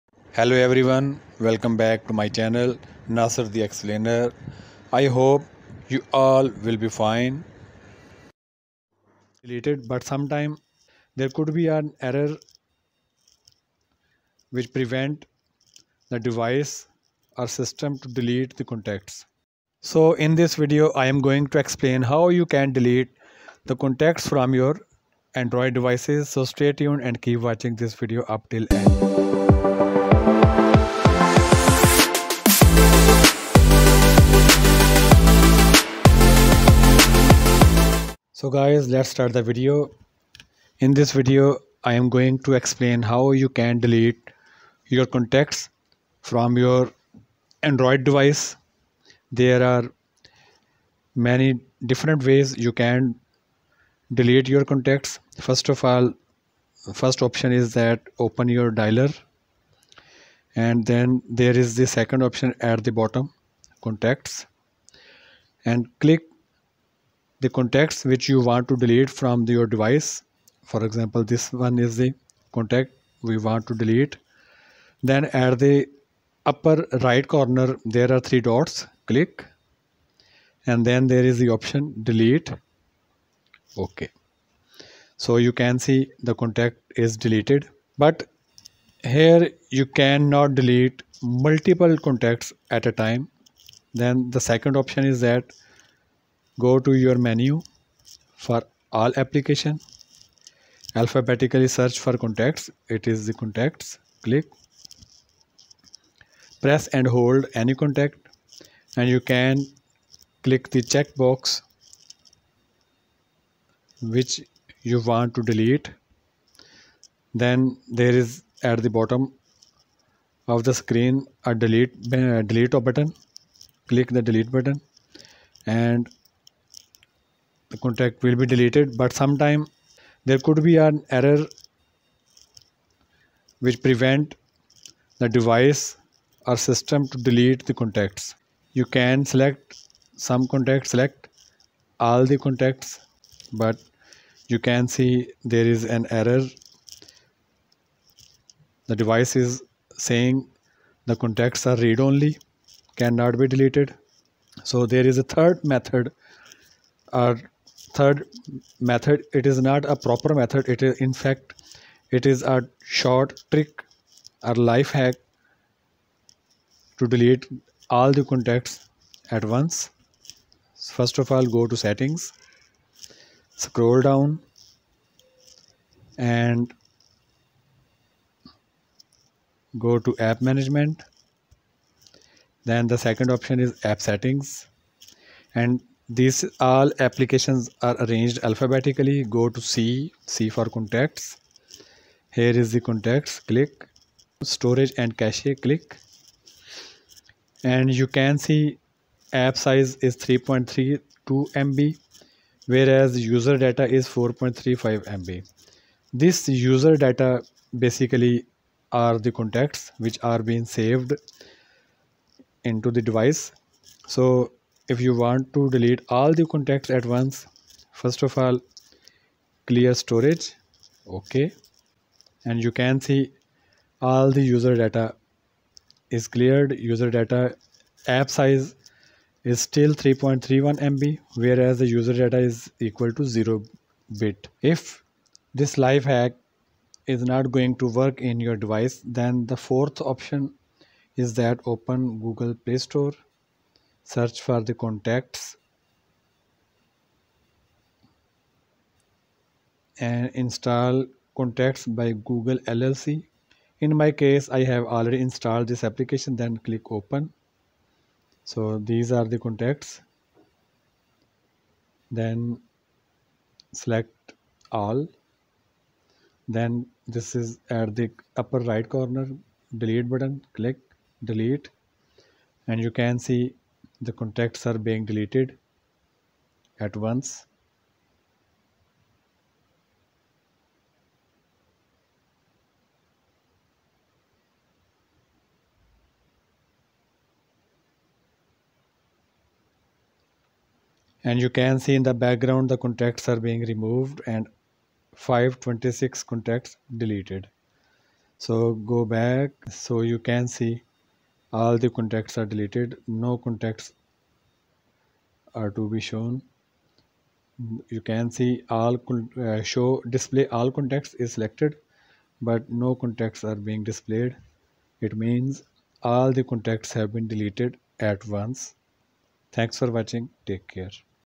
How to delete saved contacts one by one or all at once from your Android devices?
it's an educational and informational video containing the following queries:

1. how to delete contacts?
2. how to delete contacts one by one?
3. how to delete all the contacts at once?
4. how to delete contacts which are read only?
5. how to solve the error of read only contacts?
6. what are read only contacts?
7. life hack to delete all the contacts.